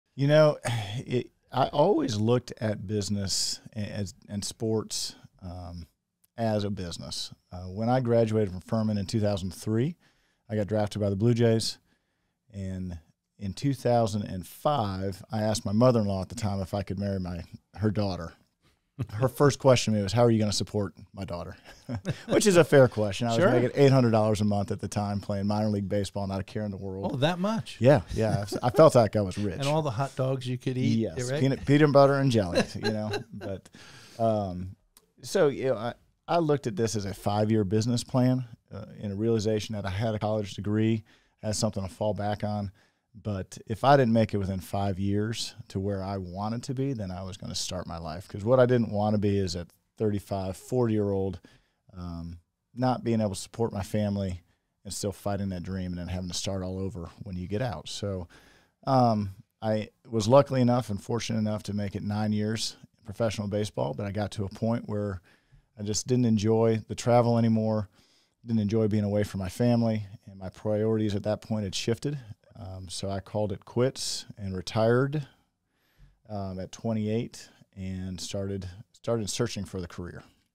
Business and Sports Not So Different
Neil sits down with Tom Mastny of The Pennant Group to talk about how health benefits work for your business.

Be sure to read more from Tom and visit our